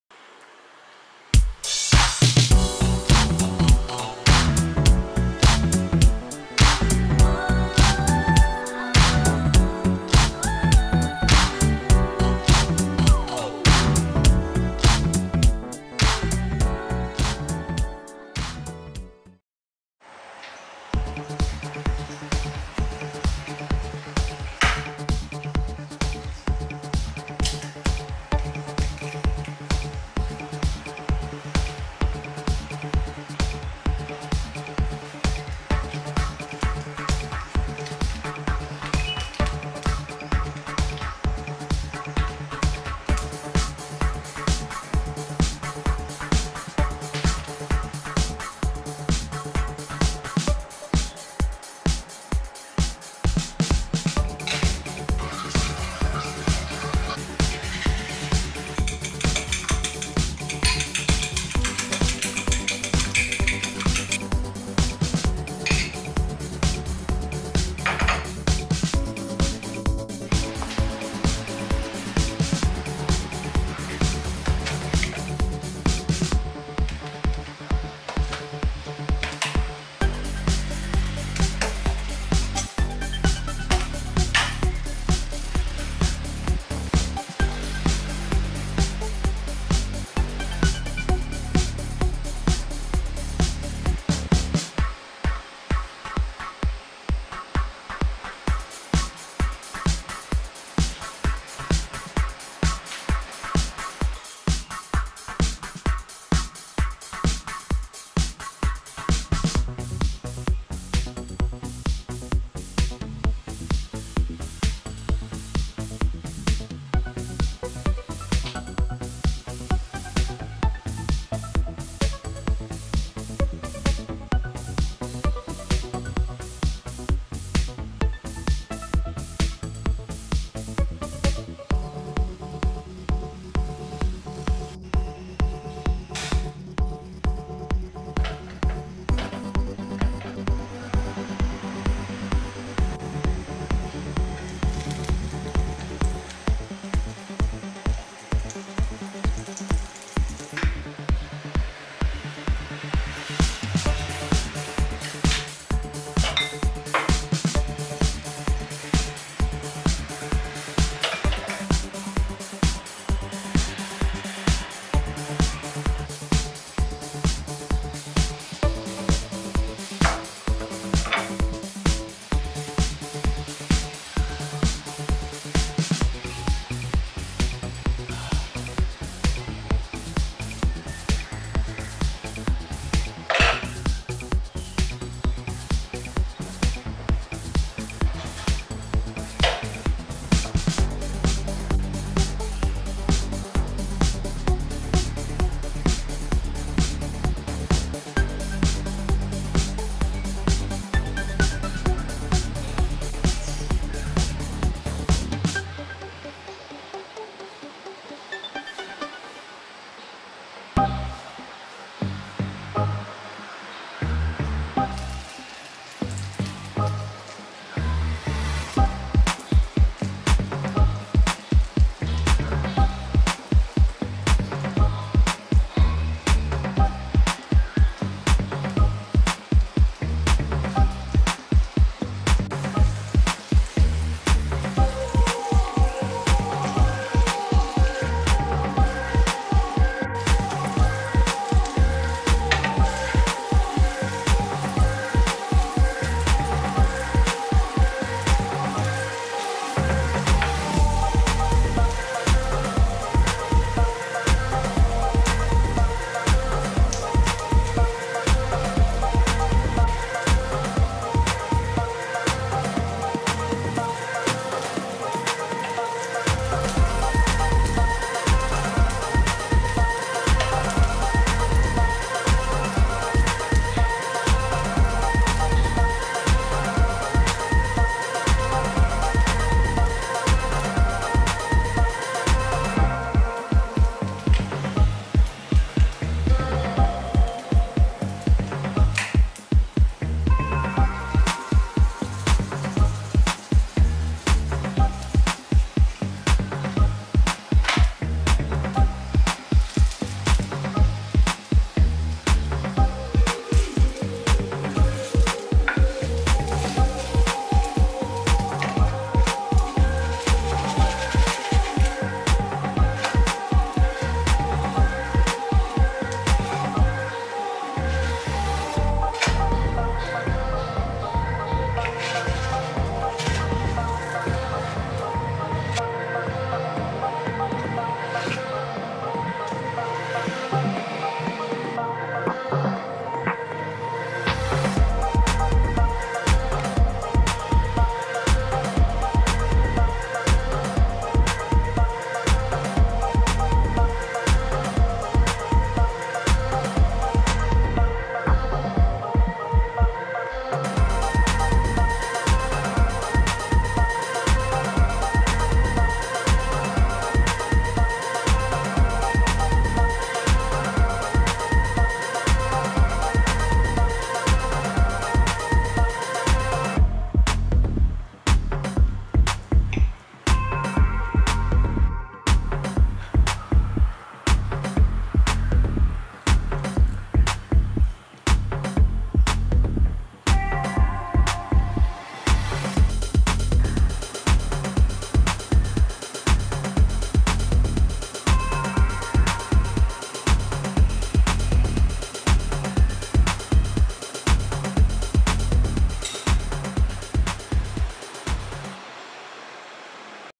Cool Flip Grilled Cheese + Egg Sandwich || Self-Love 💗 Self-Care
No more sandwich falling apart when you take a bite with this method. So now you can enjoy your sandwich - hassle free. 😉

Hello Luna 💕 Lovelies!

Sensory Self-Care - Nothing is as healing as preparing your own meal. It sparks all your five senses when you're cooking and even more so when you're eating - it gets better if you're cooking for your loved ones. Taking your mind off everything else and focus solely on cooking is the most peaceful experience ever. I hope that you would cook and nourish your body with something scrumptious today.

Happy Flipping Sandwiches!

P.S: Please comment 💡 if you're inspired to try this or ❤️ if you find this yums!

XXX,
Luna